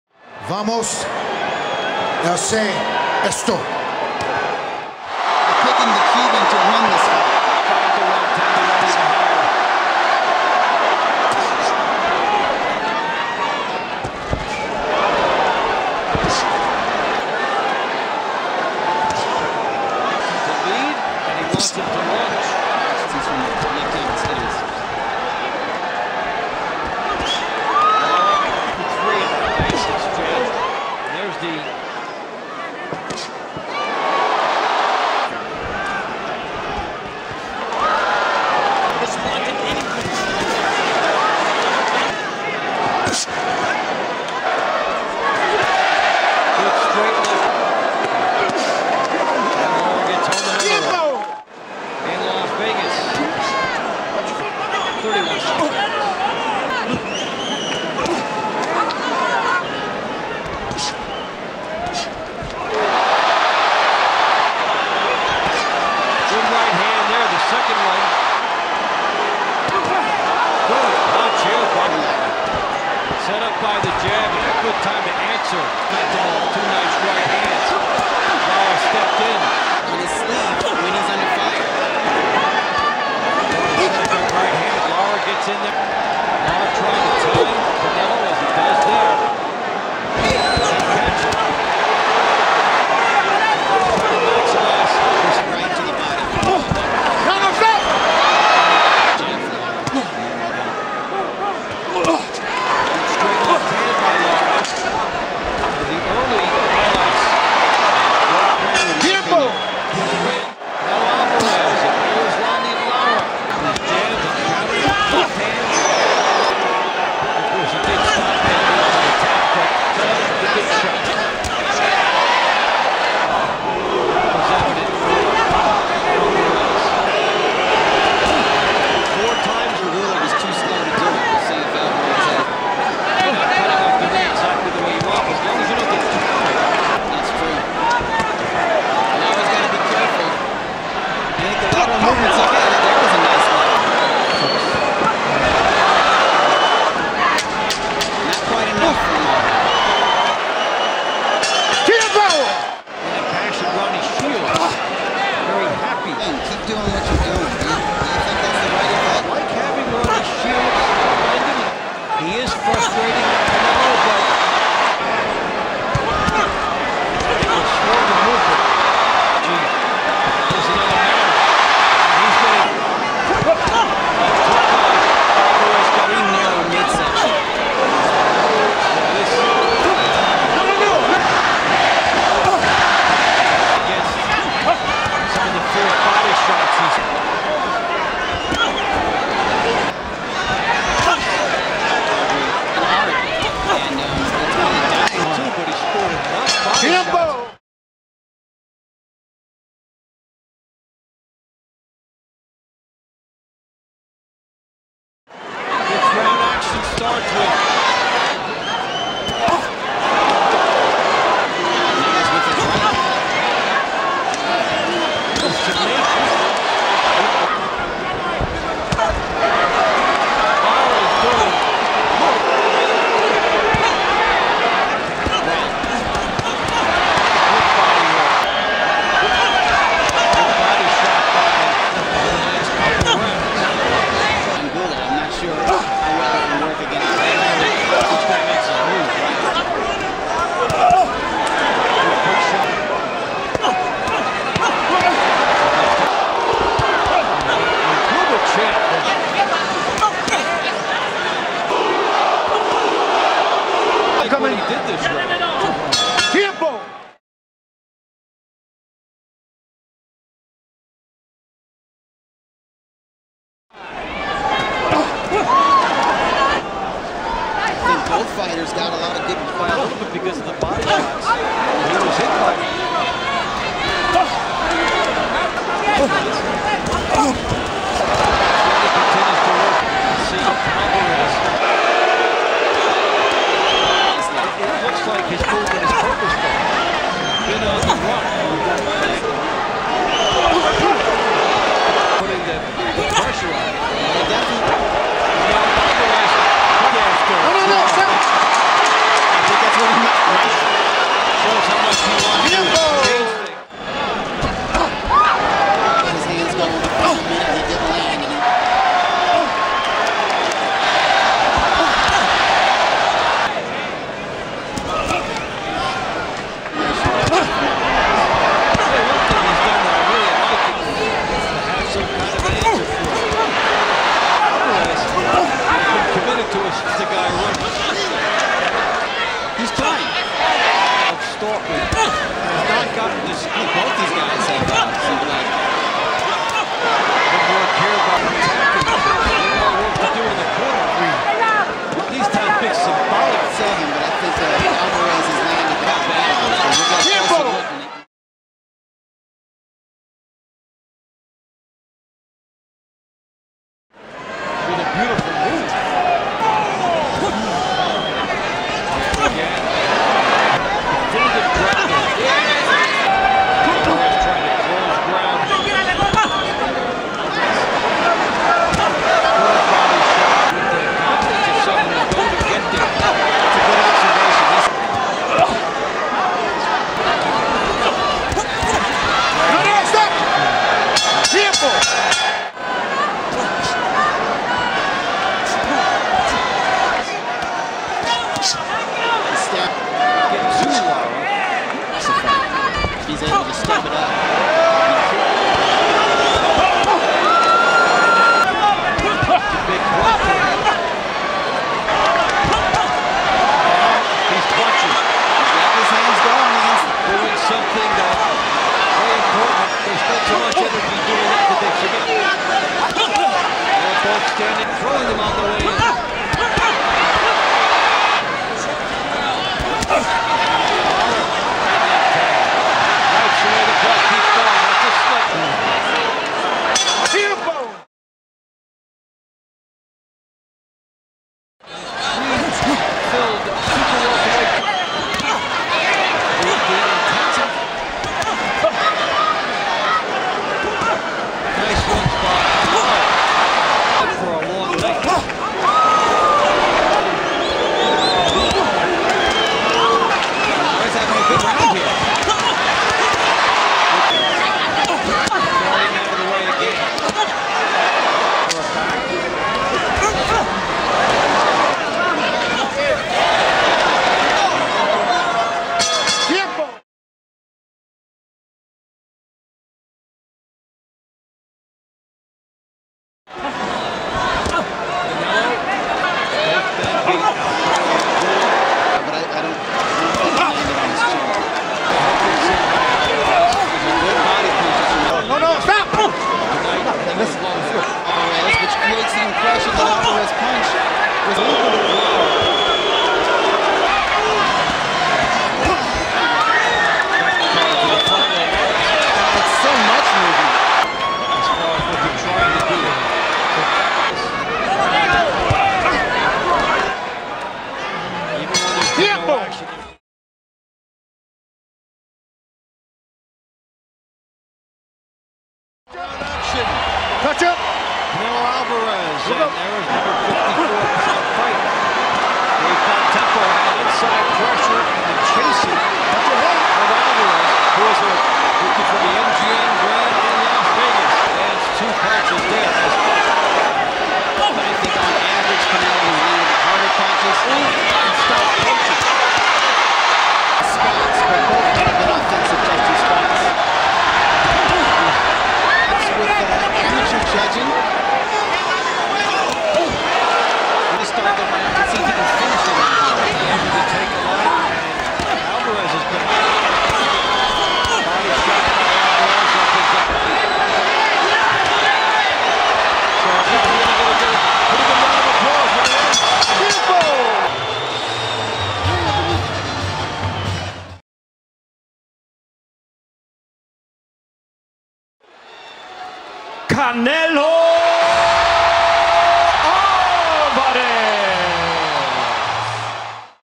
Canelo Alvarez vs Erislandy Lara FULL FIGHT HIGHLIGHTS | BOXING FIGHT HD
Приватбанк -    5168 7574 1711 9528